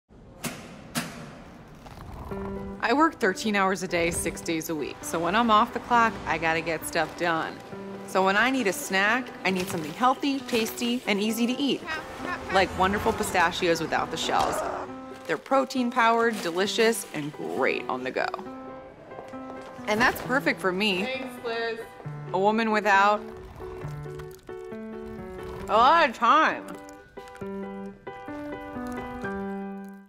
Venus de Milo on the go
Venus is a busy lady, so she prefers her pistachios as naked as she is.